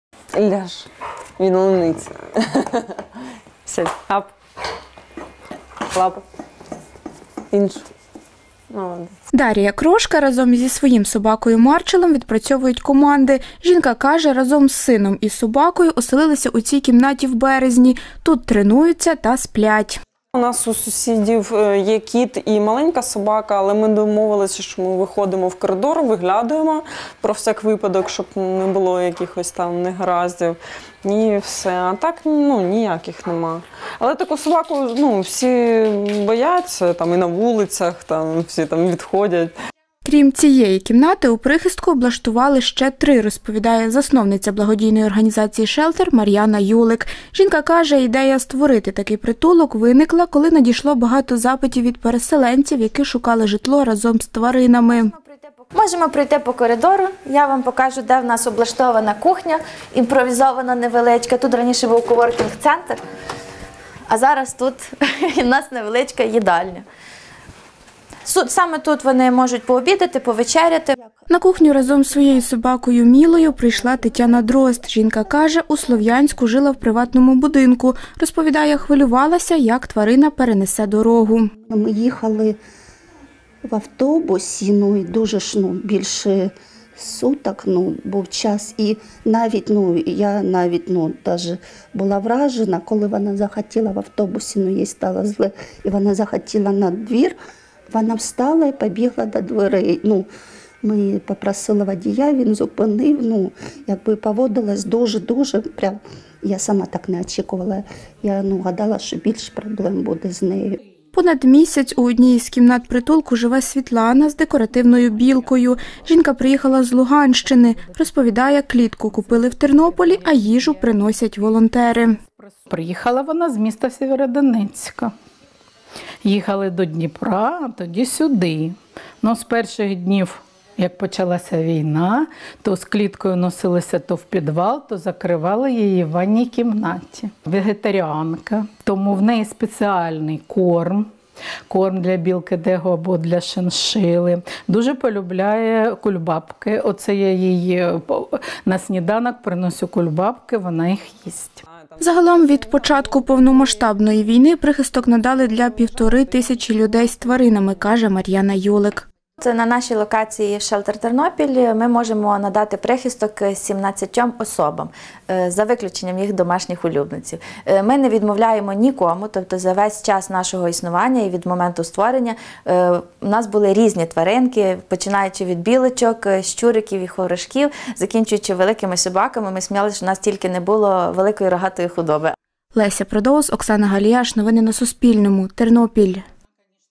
У Тернополі діє прихисток для людей та їхніх домашніх тварин
Його облаштували в колишньому офісі на вулиці Коперника. Там є 4 кімнати. Зараз у притулку – менше 10 людей і 4 тварини. Це собаки, кішка та білка.